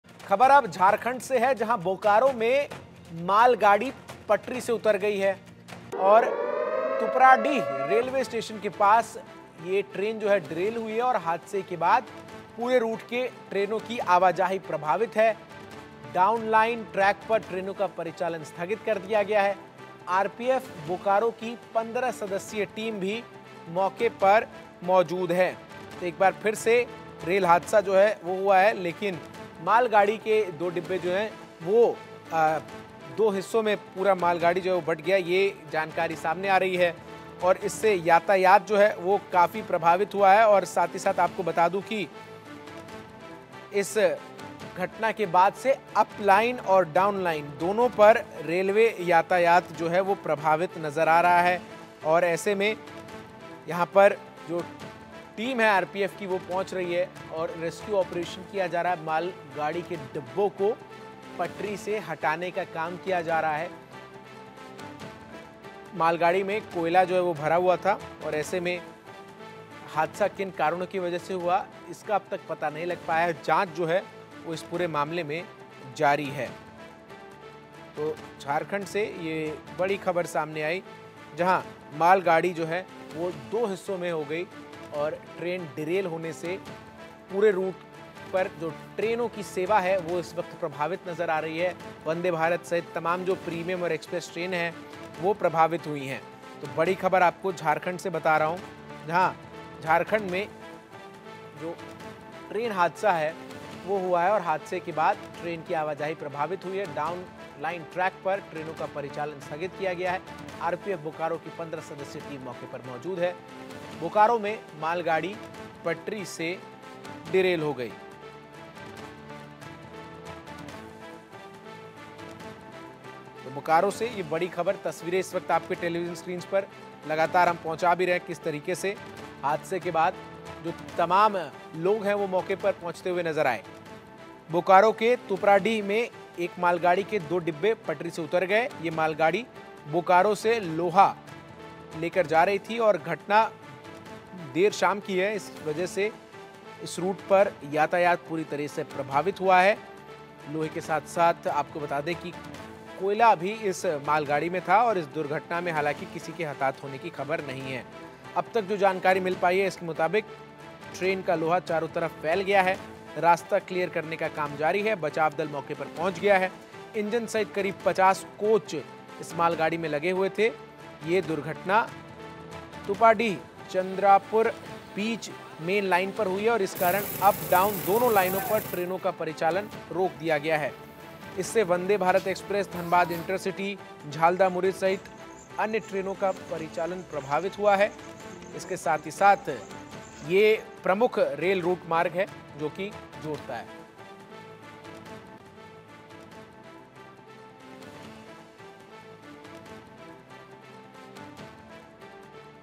Jharlkhand Breaking News: झारखंड के बोकारो में मालगाड़ी पटरी से उतरी | India News
झारखंड के बोकारो में एक मालगाड़ी गुरुवार को हादसे का शिकार हो गई। मालगाड़ी अचानक पटरी से उतर गई इसके बाद मालगाड़ी के दो डिब्बे भी पलट गए। गुरुवार सुबह हुए इस हादसे के बाद ट्रेक पर आवाजाही बाधित है। रेलवे अधिकारियों का कहना है कि रिस्टोरेशन का वर्क जारी है |

A train met with an accident in Bokaro, Jharkhand on Thursday. The goods train suddenly derailed, after which two coaches of the goods train also overturned. After this accident on Thursday morning, traffic on the track is disrupted. Railway officials say that restoration work is going on.